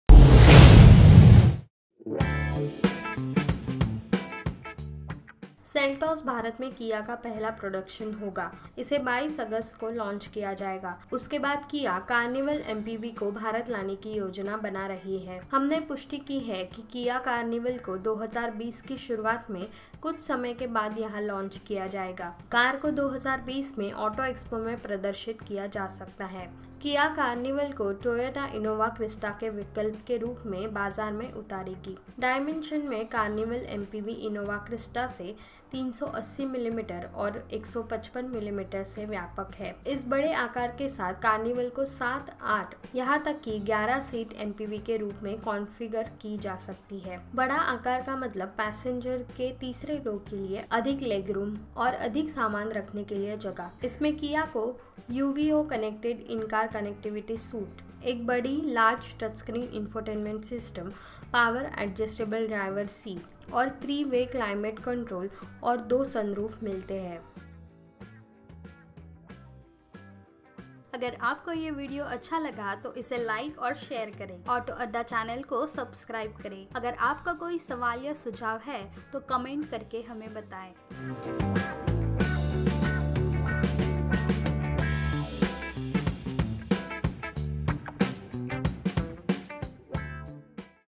Kia Carnival MPV २०२० में भारत में होगी लॉन्च
सेल्टोस भारत में किआ का पहला उत्पाद होगा। इसे २२ अगस्त को लॉन्च किया जाएगा। उसके बाद किआ कार्निवल एमपीवी को भारत लाने की योजना बना रही है। हमने पुष्टि की है कि किआ कार्निवल को २०२० की शुरुआत में कुछ समय के बाद यहां लॉन्च किया जाएगा। कार को २०२० में एक्सपो में प्रदर्शित किया जा सकता है।

किआ कार्निवाल को टोयोटा इनोवा क्रिस्टा के विकल्प के रूप में बाजार में उतारेगी। डायमेंशन में, कार्निवाल एमपीवी इनोवा क्रिस्टा से ३८०मिमी और १५५ मिमी से व्यापक है। इस बड़े आकार के साथ, कार्निवल को सात, आठ और यहां तक कि ग्यारह सीटर एमपीवी के रूप में कॉन्फ़िगर कि जा सकती है।बड़ा आकार का मतलब यात्रियों की तीसरी पंक्ति के लिए अधिक लेग रूम और अधिक सामान स्थान है। इसमें किआ का यूवीओ कनेक्ट इन-कार कनेक्टिविटी सूट, एक बड़ा टचस्क्रीन इंफोटेनमेंट सिस्टम, पावर एडजस्टेबल ड्राइवर की सीट, तीन तरह का क्लाइमेट कंट्रोल और दो सनरूफ जैसी चीजें मिलेंगी।